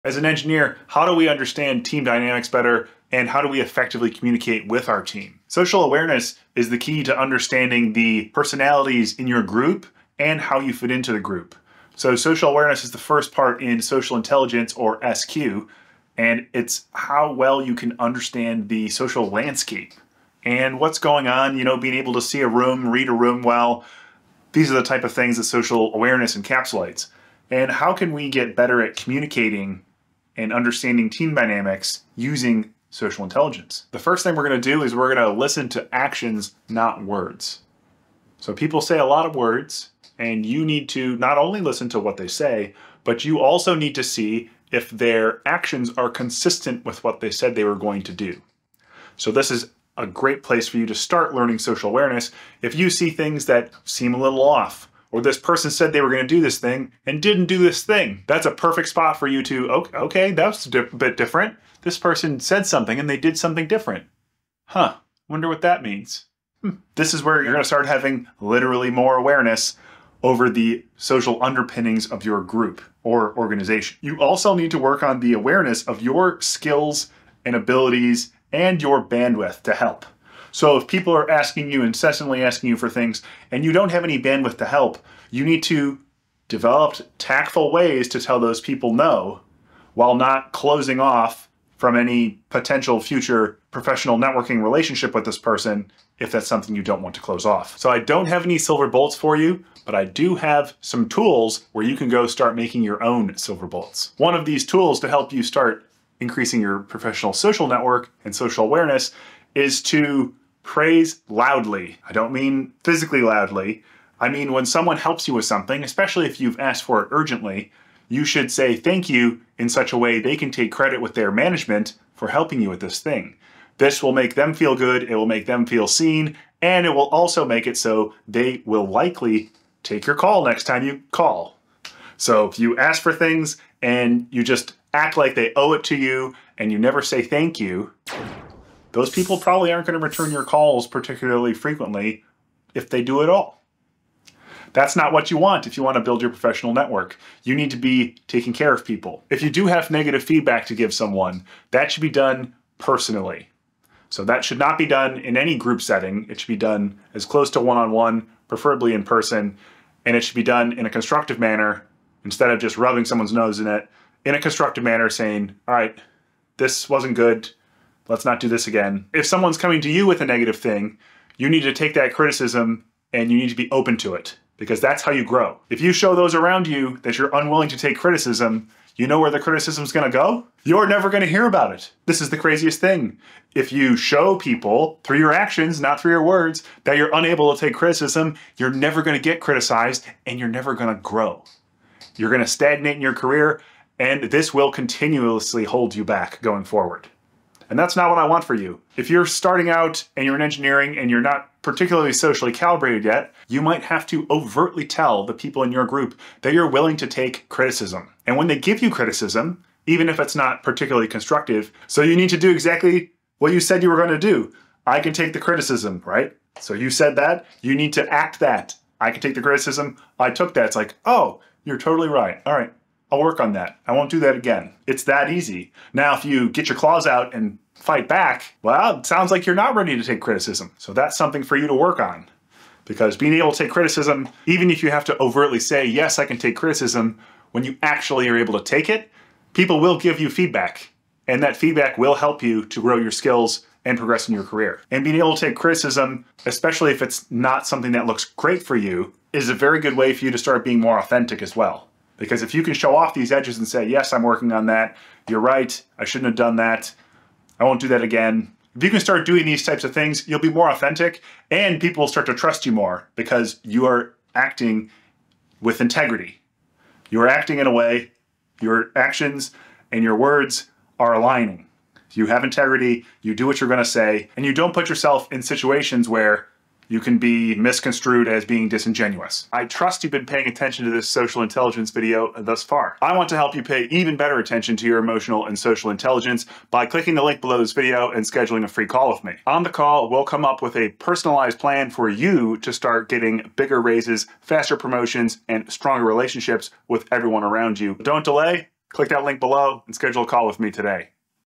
Navigating Team Dynamics as an Engineer (Tips for Effective Collaboration)
Learn how to navigate team dynamics effectively as an engineer with these valuable tips on collaboration and communication.

Chapters:
00:00:00 Intro
00:00:33 Listen to actions not words
00:01:44 Praise loudly
00:02:55 If you can't take feedback, you will not grow